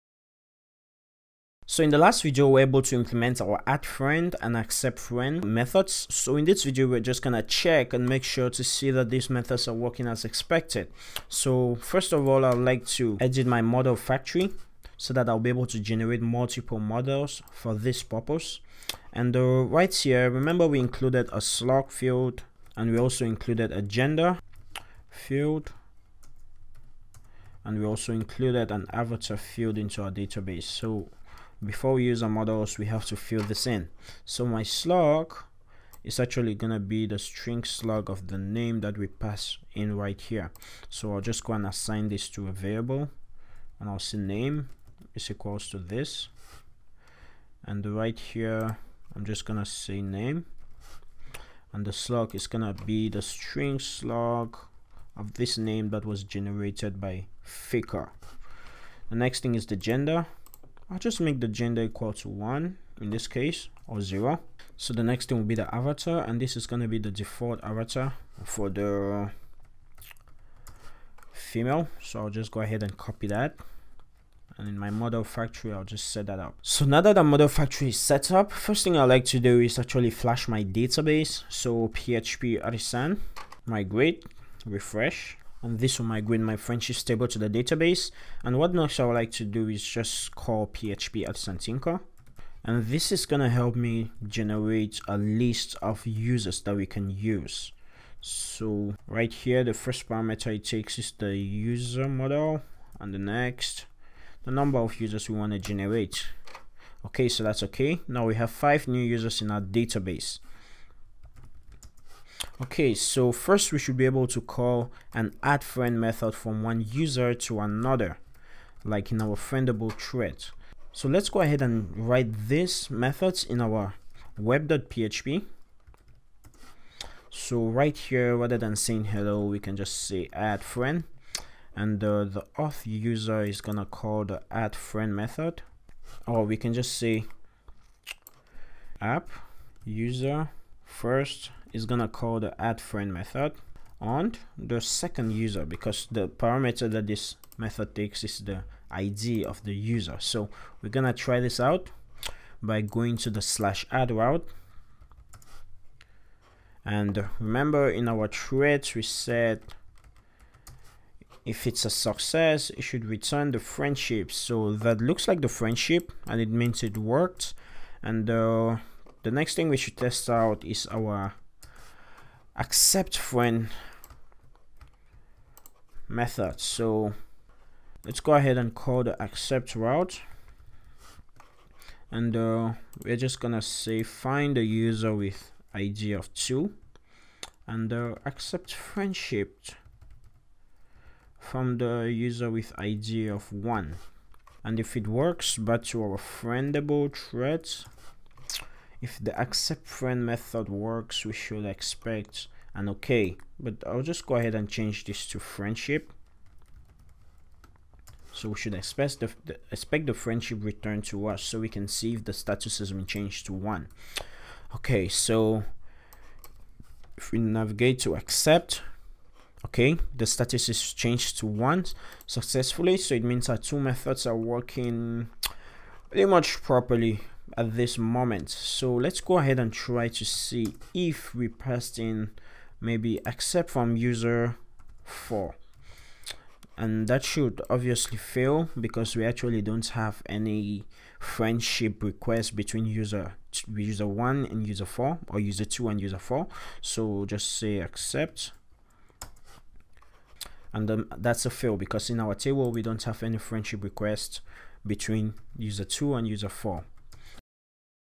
Social network in Laravel 5.3 and Vuejs 2.0 #11 - Testing add_friend and accept_friend methods
Let's build a complete application with the Laravel 5.3 and Vuejs 2.0. In this video, we tested out our accept_friendship and add_friend methods.